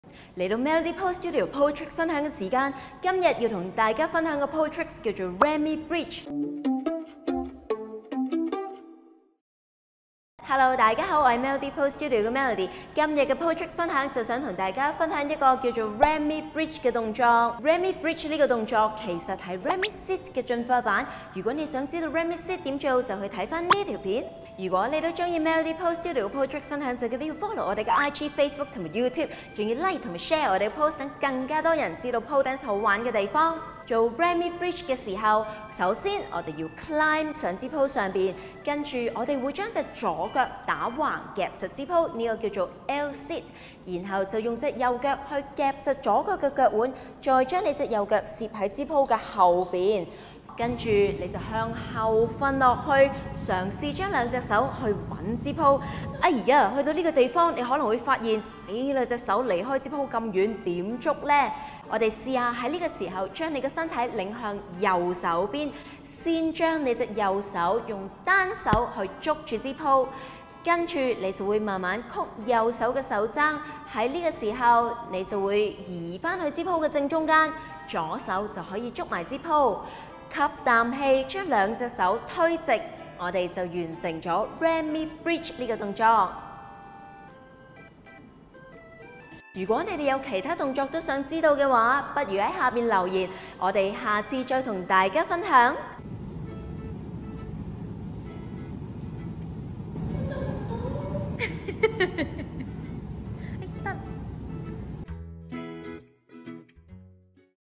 【Pole Dance教室】Remibridge || 鋼管舞 || POLE TRICKS
今次同你地講下Remibridge依個動作，Remebrigde睇落去好似好難😵‍💫
但係佢仲容易過layback😆只不過係好痛姐～haha！
唔怕痛既你地就可以試下嫁～